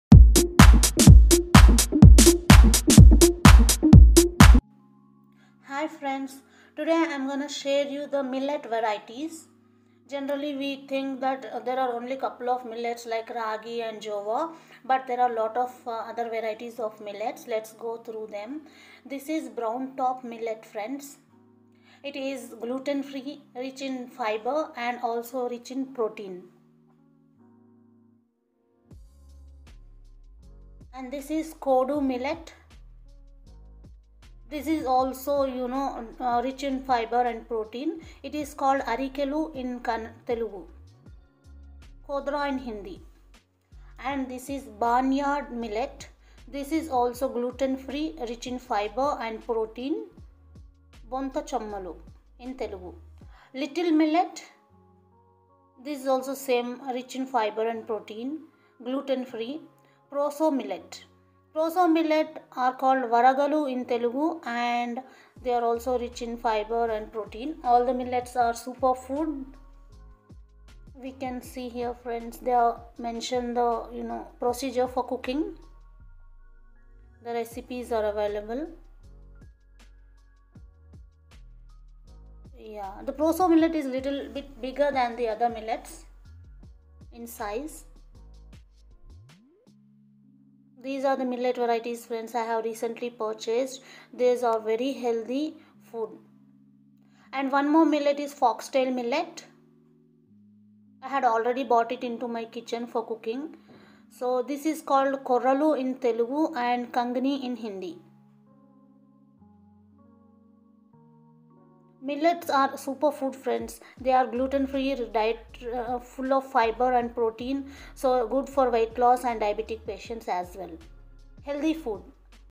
Millet Varieties|Weight loss food Millet grain varieties|Gluten free, High Fibre,High protein grain
Millet Varieties| All Millet grain varieties explained| Gluten free, High Fibre, High protein grain

Super Food- Millets